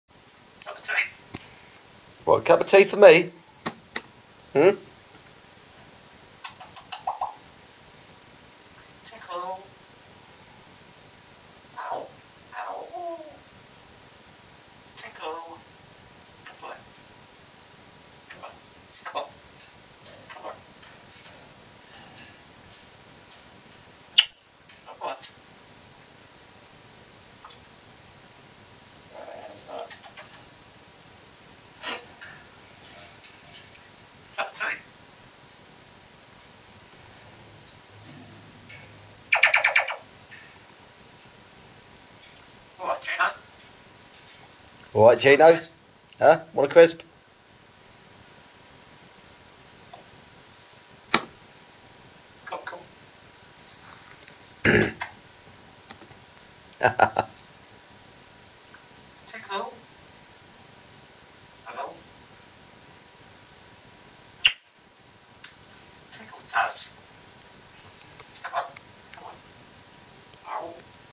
GINO THE AFRICAN GREY COCKNEY
COCKNEY AFRICAN GREY PARROT CALLED GINO